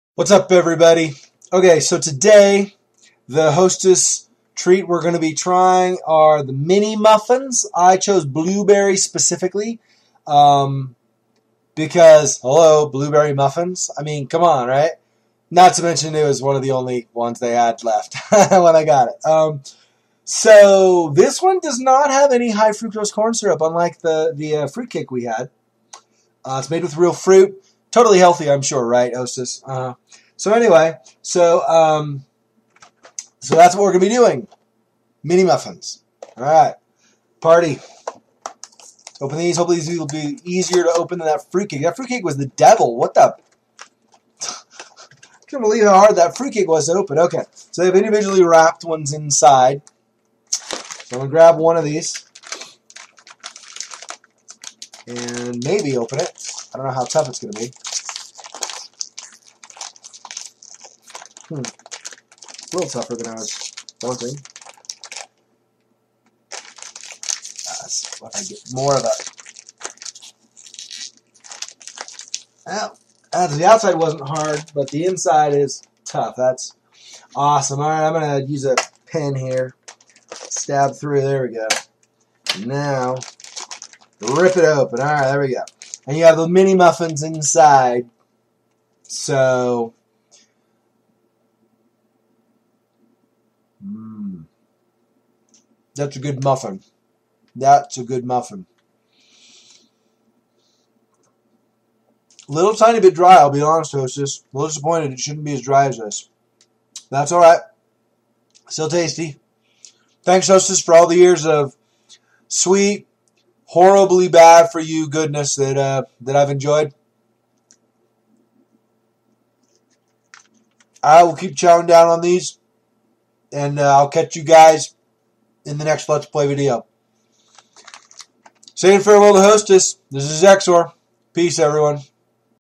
Last Days of Hostess, Day 3: Mini Muffins
On November 16, 2012, Hostess Brands announced they would be closing down for good.  Between financial woes that plagued them for years and a bakers strike, they not only have filed for bankruptcy (again), but they also will be selling off parts of their business.  Here's to nearly 83 years of sweet cakes, soft breads, and delicious deserts! :)